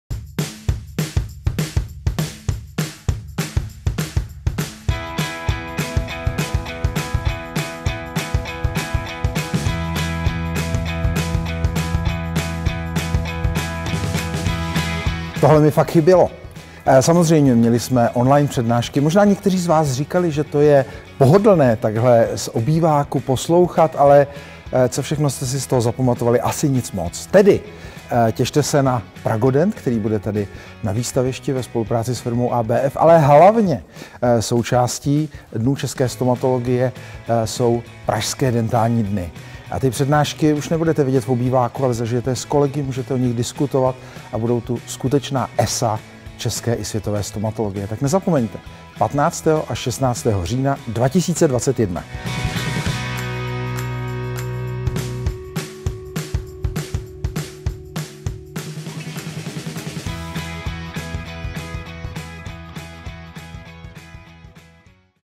Pozvánka na Pražské dentální dny 2021
Prezident České stomatologické komory MUDr. Roman Šmucler CSc. zve na Pražské dentální dny, které se konají společně s veletrhem PRAGODENT 2021.